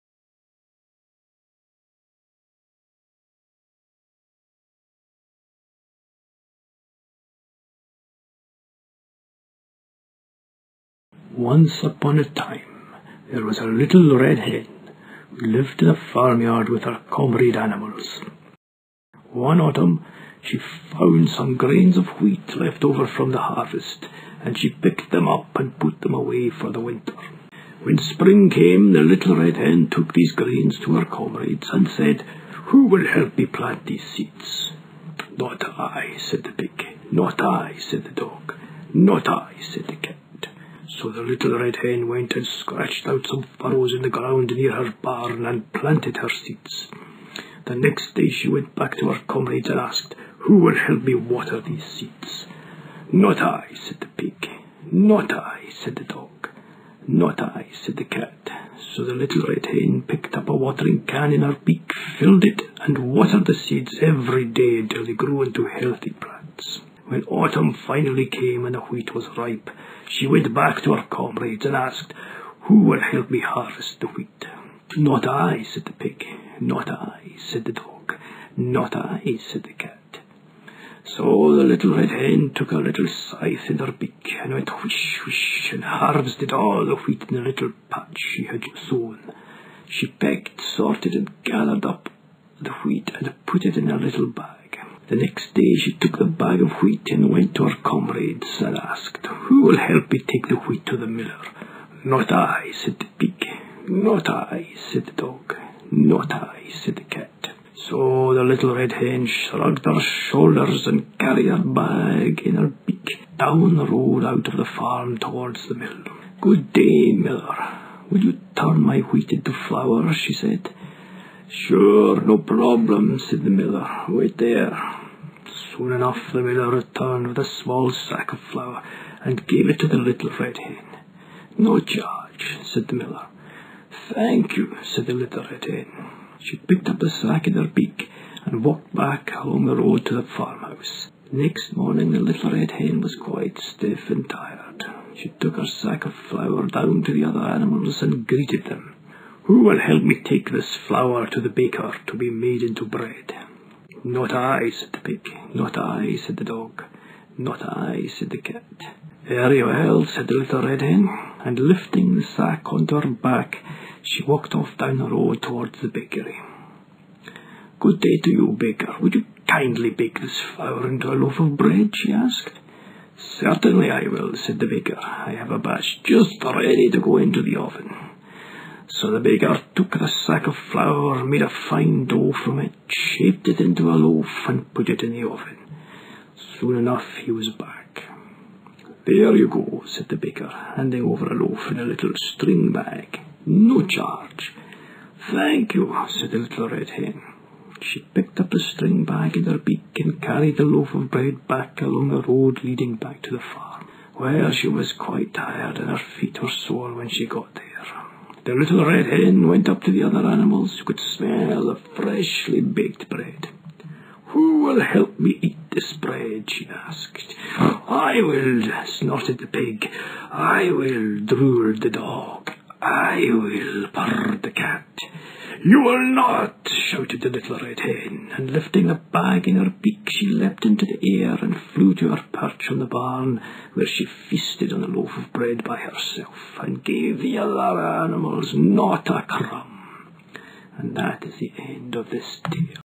The Little Red Hen
An animated version of the folk tale featuring the Little Red Hen and her labours to turn some seeds into a loaf of bread.

Created in Adobe Flash CS5 using its inverse kinematic bones features; with some household textures. Styled with Soviet animated propaganda touches in mind.